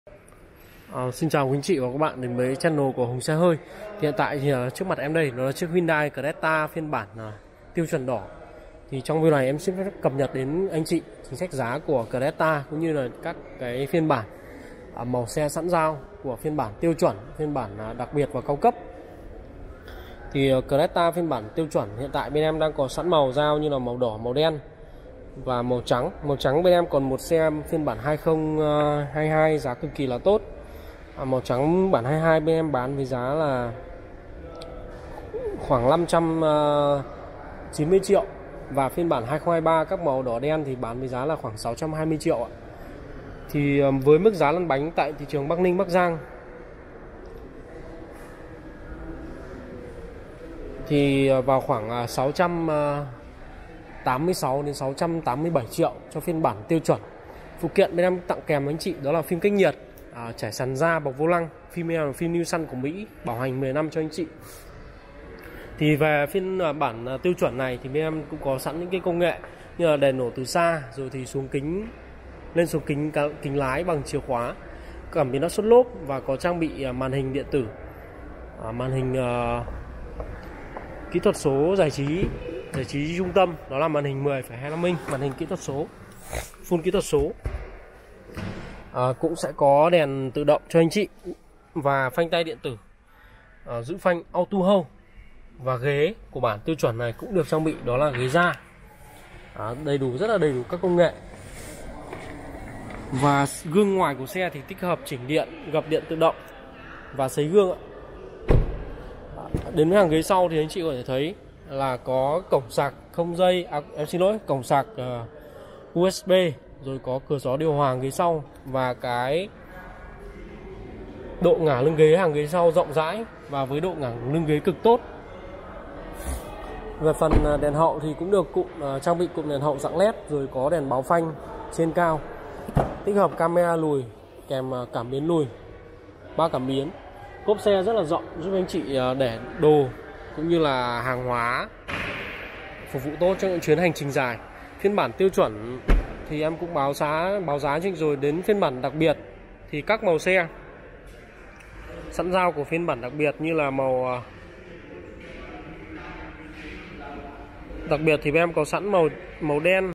Giá Xe Hyundai Creta Tháng 3 - 2023 | Khuyến Mại, Lăn Bánh Tại Bắc Ninh | Vẫn Xả Kho 2022 590 Triệu
+ Hotline Tư Vấn: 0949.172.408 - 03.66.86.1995